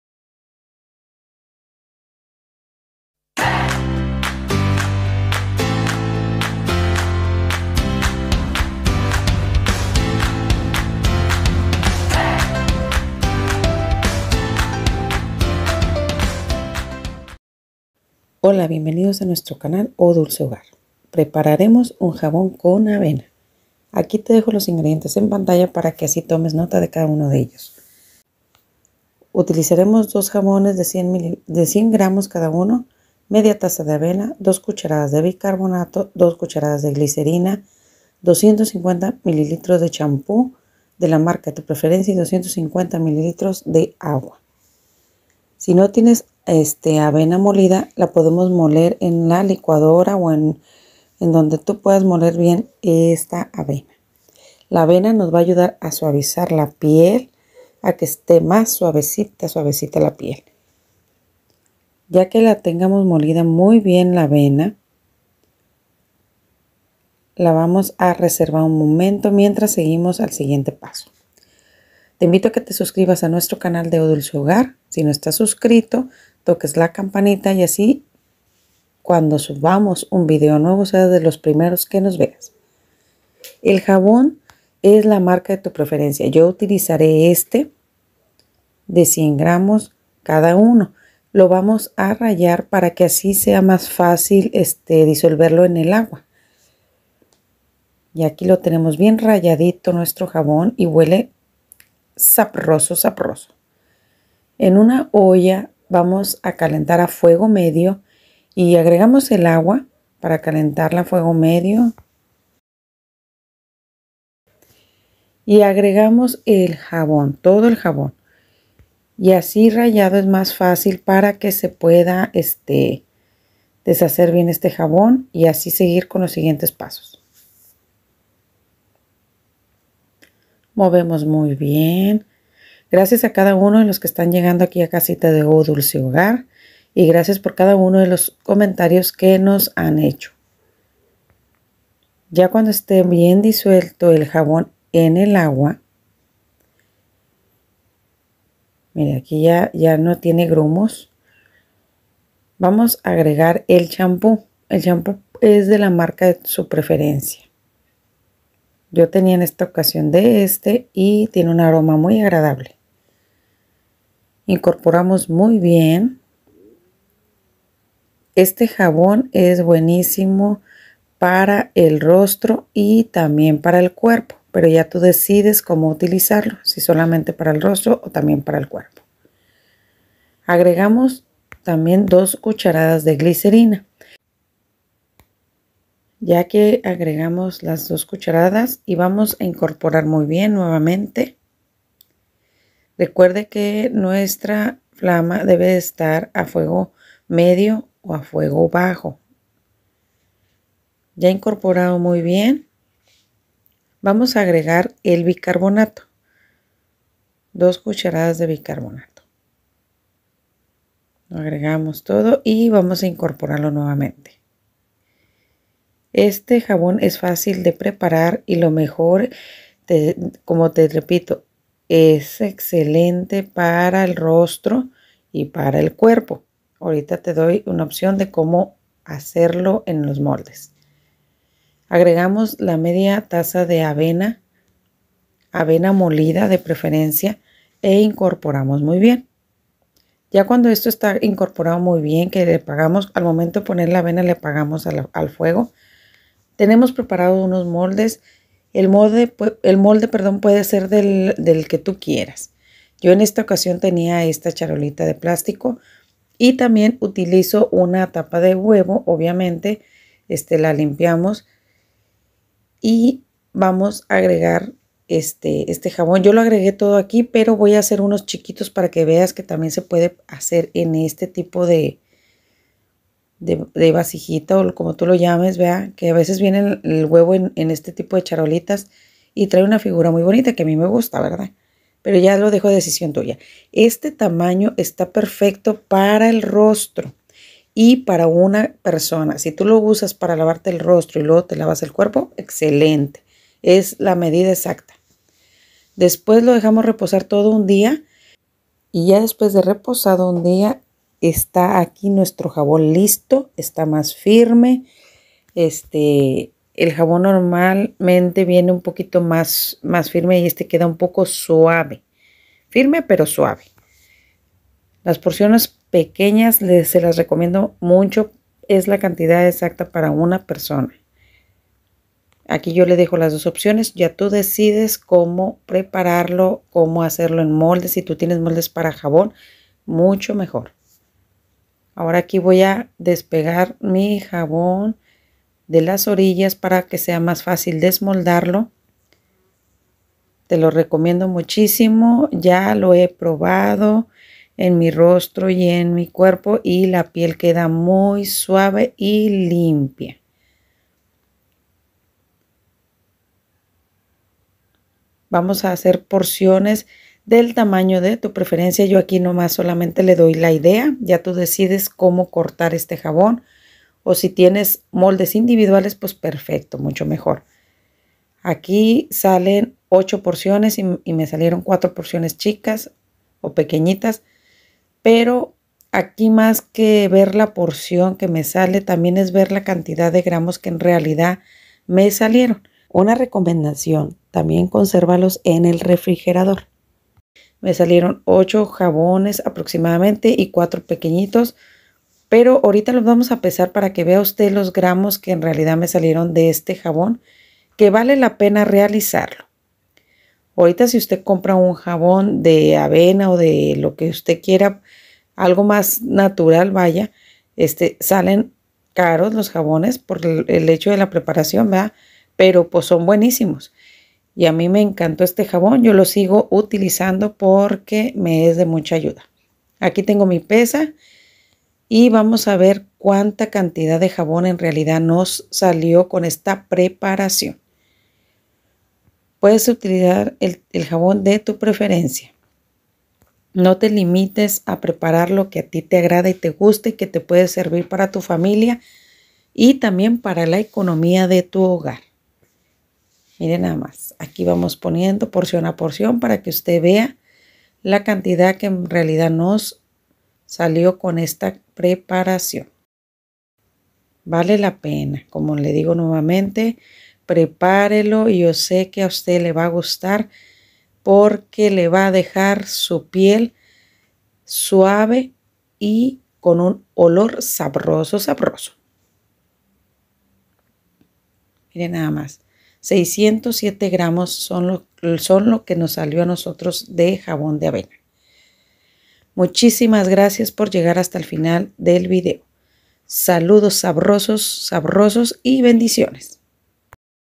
JABÓN CON AVENA
INGREDIENTES

2 jabones de 100 grs. c/ u
1/2 tz Avena  molida
2 cds de Bicarbonato
2 cds de Glicerina
250 ml. de Shampoo
250 ml. de Agua

RECOMENDACIONES
LOS JABONES YA PREPARADOS CONSERVALOS  EN EL REFRIGERADOR.

Usalo para limpieza de cara o cuerpo,  espero y te guste y sea de ayuda en la economía de tu hogar,  saludos sabrrrossos y bendiciones 👍 😊